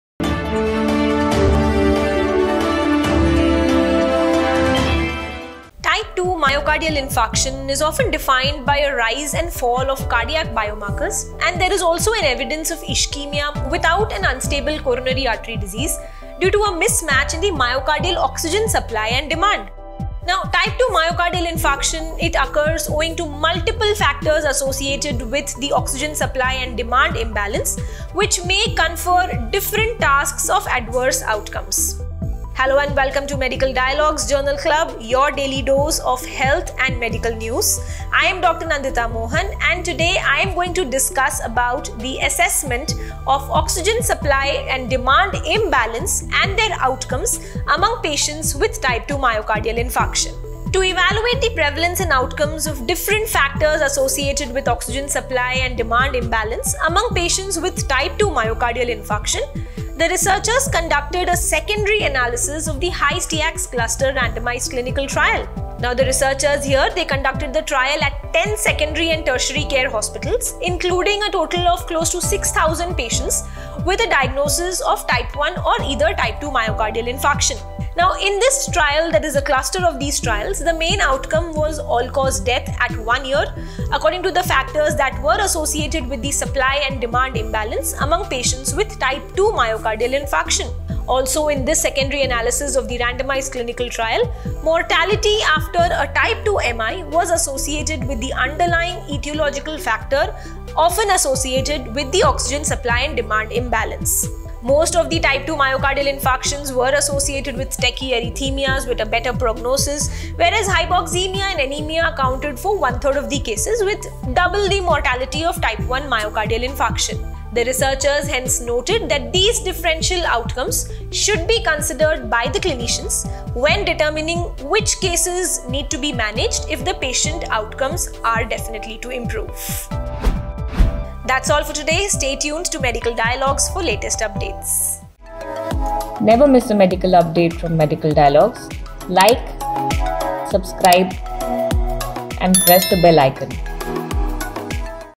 Oxygen Supply-Demand Imbalance may be tied to Type 2 Myocardial Infarction.
Type 2 myocardial infarction (MI) is often defined by a rise and fall of cardiac biomarkers and evidence of ischemia without unstable coronary artery disease (CAD), due to a mismatch in myocardial oxygen supply and demand.
Type 2 myocardial infarction occurs owing to multiple factors associated with myocardial oxygen supply-demand imbalance, which may confer different risks of adverse outcomes.